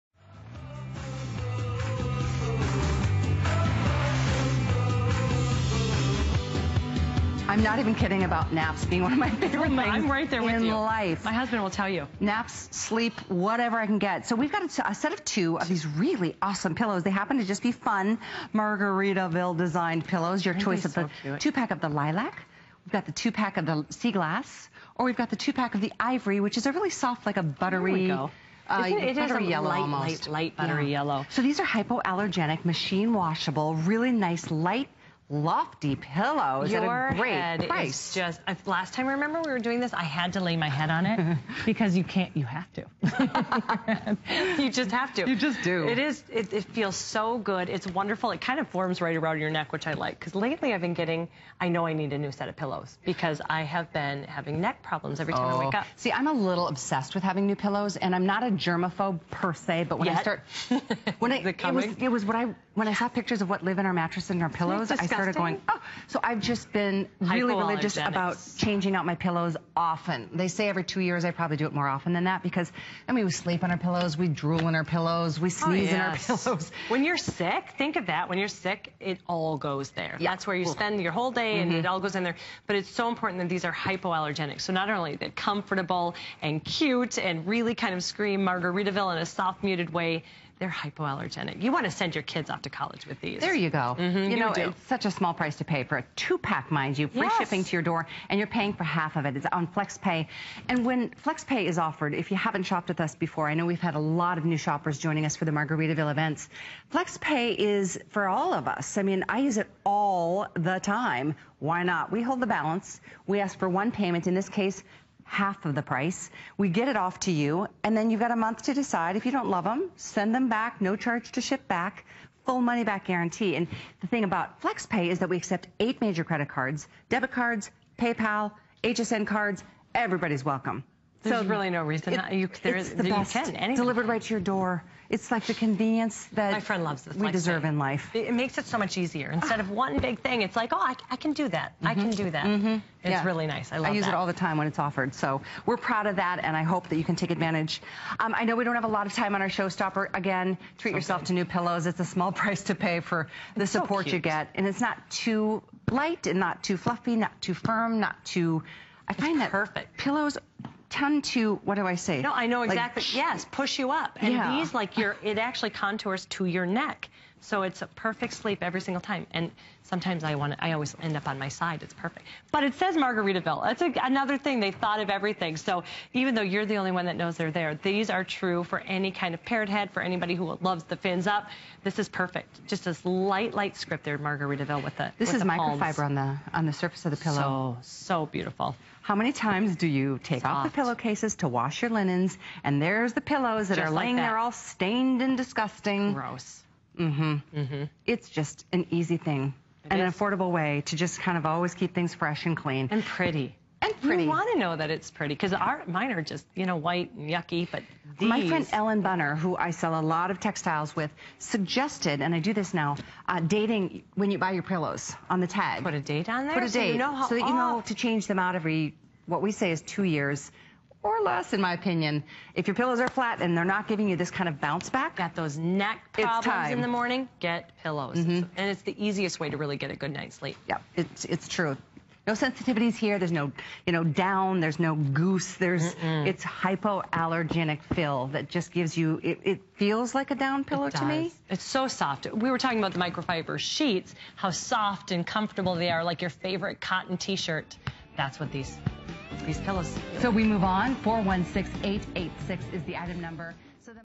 Margaritaville Palm Leaves Bed Pillows 4pack
Chill out with these pretty pillows. Palm prints, full of visual movement create a paradise destination with big, billowing leaves flowing in the...